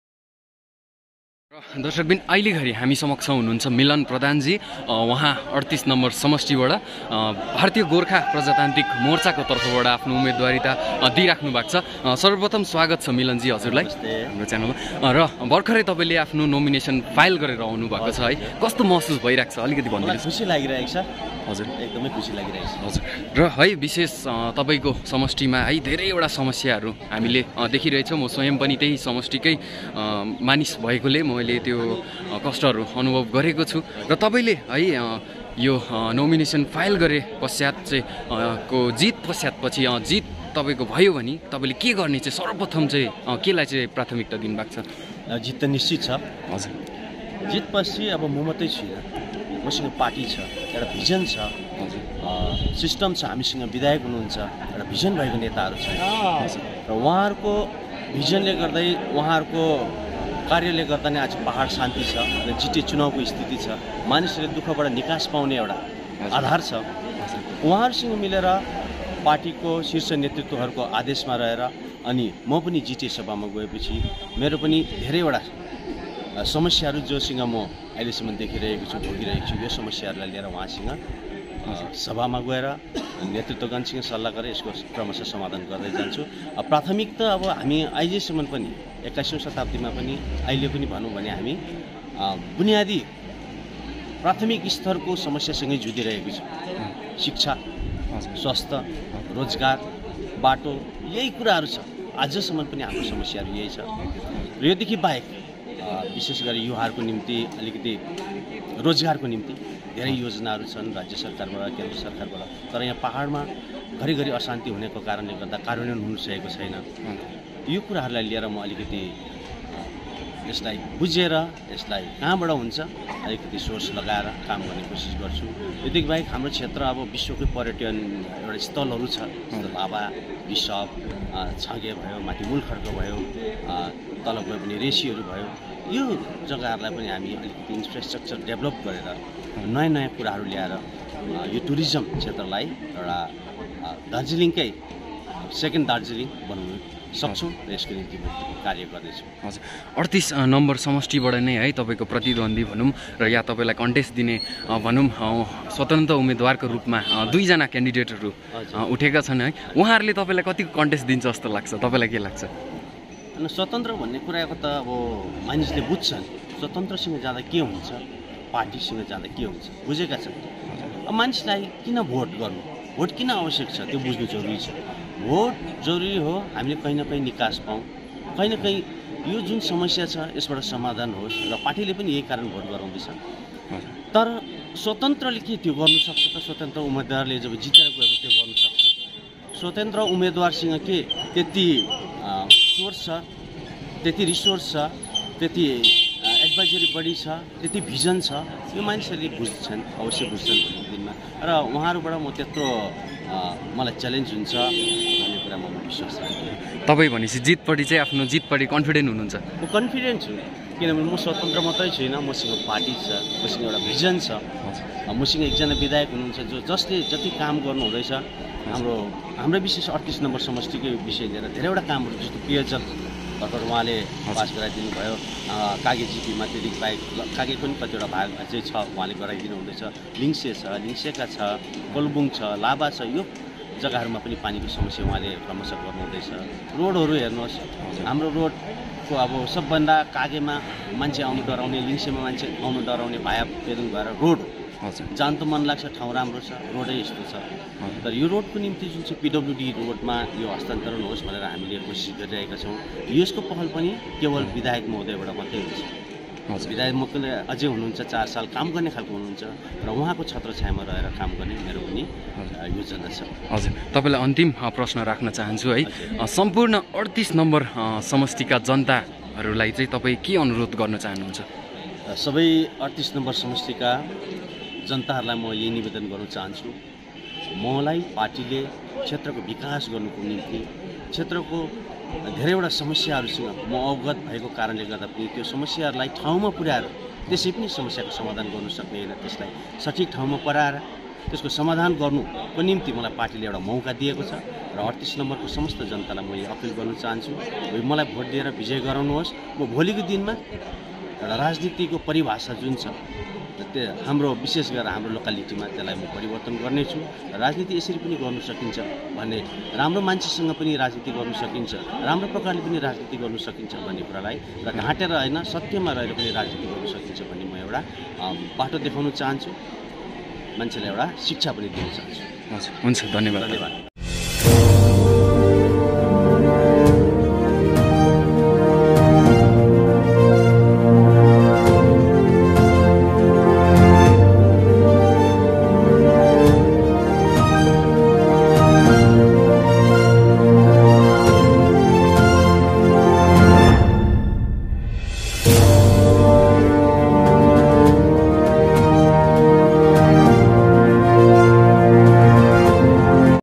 जीत निश्चित छ, पार्टीको भिजन अनुसार जीटीएको बिकास मार्फत आफ्नो क्षेत्रको समस्याको समाधान गर्छु
जीत निश्चित छ, पार्टीको भिजन अनुसार जीटीएको बिकास मार्फत आफ्नो क्षेत्रको समस्याको समाधान गर्छु- मिलन प्रधान

कालेबुङ,२ जुन २०२२ (निंस)
जीटीए चुनाउको निम्ति भागोप्रमो ३८ नम्बर समष्टिका उम्मेदवार मिलन प्रधानले आज भरे आफ्नो नामाङ्कन पत्र।नामाङ्कन दर्ता पछि पत्रकारसम्मेलनमा बोल्दै प्रधानले भने मेरो जीत निश्चित छ, पार्टीको भिजन अनुसार जीटीएको बिकास मार्फत आफ्नो क्षेत्रको समस्याको समाधान गर्छु | Gorkhaland Flag Campaign